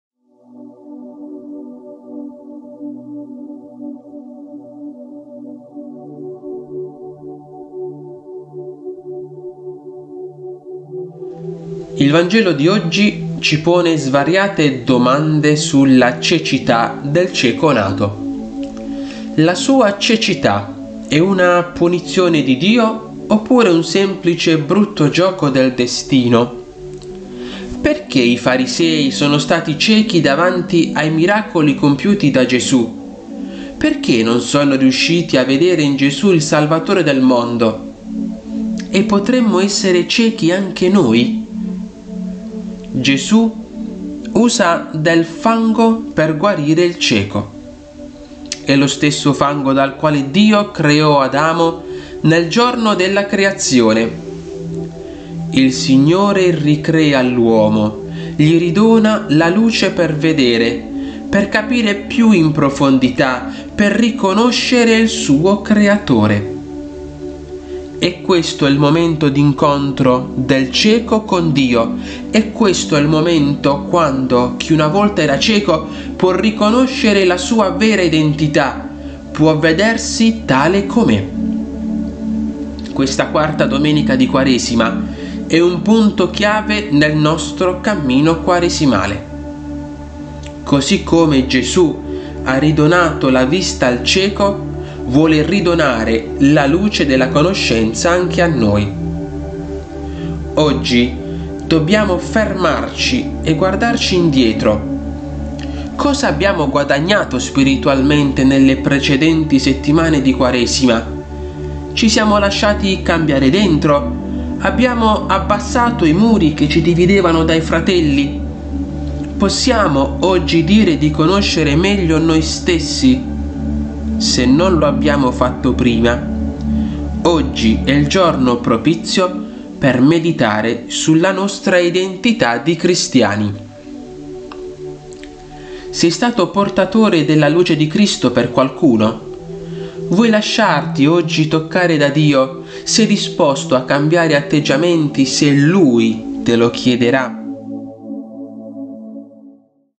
⏩ PAROLE DI QUARESIMA 2023 | 19 marzo
Domenica 19 marzo

📖 Gv 9,1-41

Il Vangelo di oggi ci pone svariate domande sulla cecità del cieco nato. La sua cecità è una punizione di Dio oppure un semplice brutto gioco del destino?
Perché i farisei sono stati ciechi davanti ai miracoli compiuti da Gesù? Perché non sono riusciti a vedere in Gesù il Salvatore del mondo? Potremmo essere ciechi anche noi?
Gesù usa del fango per guarire il cieco. È lo stesso fango dal quale Dio creo Adamo nel giorno della creazione. Il Signore ricrea l’uomo, gli ridona la luce per vedere, per capire più in profondità, per riconoscere il suo Creatore.
È questo il momento d’incontro del cieco con Dio. È questo il momento quando chi una volta era cieco può riconoscere la sua vera identità, può vedersi tale come è.
Questa quarta domenica è un punto chiave sul nostro cammino quaresimale. Così come Gesù ha ridonato la vista al cieco, vuole ridonare la luce della conoscenza anche a noi.
Oggi dobbiamo fermarci e guardare indietro: cosa abbiamo guadagnato spiritualmente nelle precedenti settimane di Quaresima? Ci siamo lasciati cambiare dentro, abbiamo abbassato i muri che
ci dividevano dai fratelli? Possiamo oggi dire di conoscere meglio noi stessi?
Se non lo abbiamo fatto prima, oggi è il giorno propizio per meditare sulla nostra identità di cristiani.

Per riflettere ...
▪ Sei stato portatore della luce di Cristo per qualcuno?
▪ Vuoi lasciarti oggi toccare da Dio, sei disposto a cambiare atteggiamenti se Lui te lo chiederà?

Riflessione tratta da libro «Quaresima 2023 giorno per giorno: parola, riflessioni, proposito»